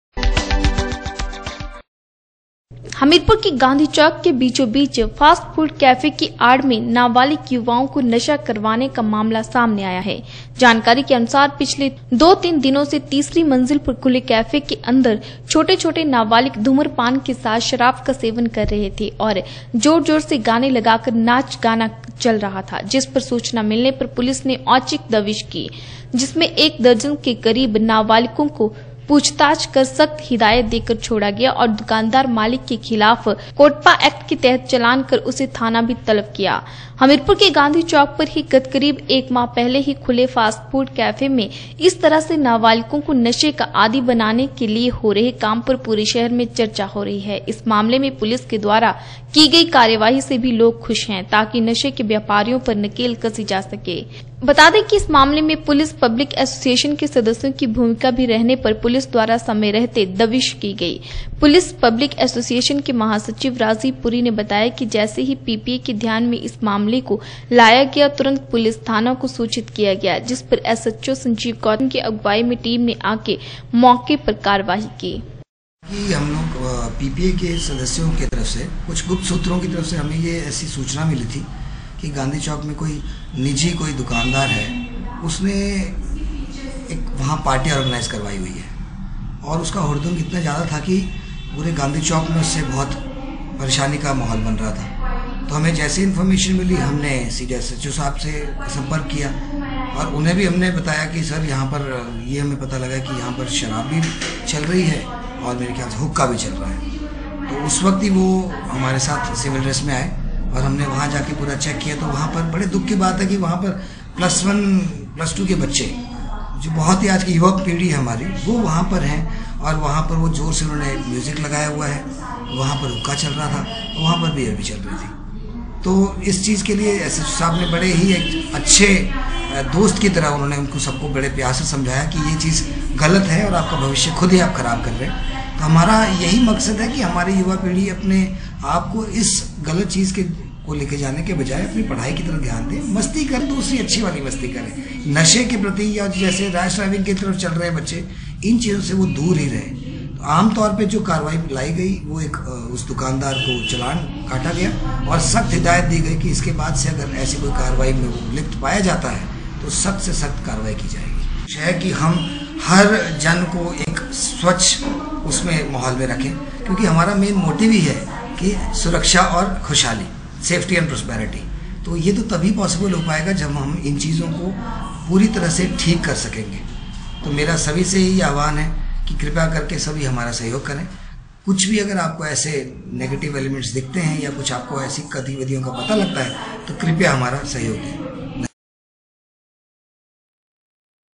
गांधी चैक के बीचों बीच फास्ट फूड कैके की आड मेंनाबालिग युवाओंको नशा करवाने का मामला सामने
हमीरपुर के गांधी चैक के बीचों बीच फास्ट फूड कैके की आड मेंनाबालिग युवाओंको नशा करवाने का मामला सामने आया है। जानकारी के अनुसार पिछले दो तीन दिनों  से तीसरी मंजिल पर खुले कैफे के अंदर छोटे छोटे नाबालिग धूम्रपान के साथ शराब का सेवन कर रहे थे और जोर जोर से गाने लगाकर नाच गाना चल रहा था जिस पर सूचना मिलनेपर पुलिस ने औचक दबिश दी जिसमें एक  दर्जन के करीब नाबालिगों को पूछताछ कर  सख्त हिदायत देकर छोडा और दुकानदार मालिक के खिलाफ कोटपा एक्ट के तहत चालान कर उसे थाना भी तलब किया।